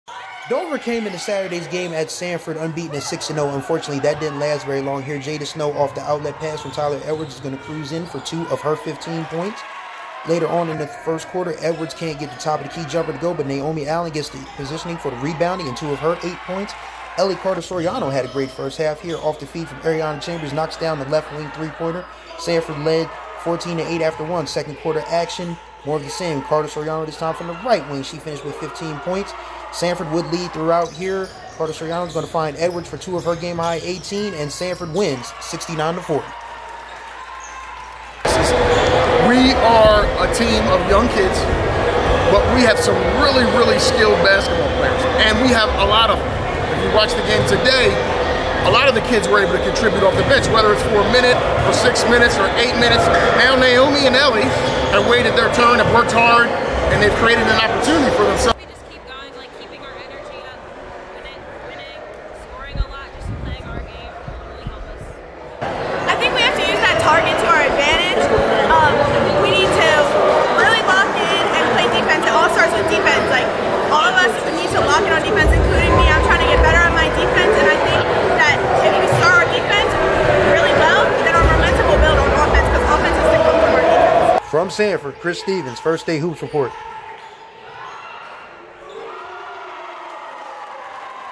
Team effort pushes Sanford girls past Dover
The Sanford girls' basketball team is a much younger team than the one that won the state championship last year, but with sharpshooting Ellie Carter-Soriano (15 points) and sophomore forward Naomi Allen crashing the boards and playing defense, it was enough for the Warriors to knock Dover high from the ranks of the unbeaten Saturday afternoon.

Interviews in order:

Sanford head coach Marcus Thompson
Sanford sophomore forward Naomi Allen
Sanford junior guard Ellie Carter-Soriano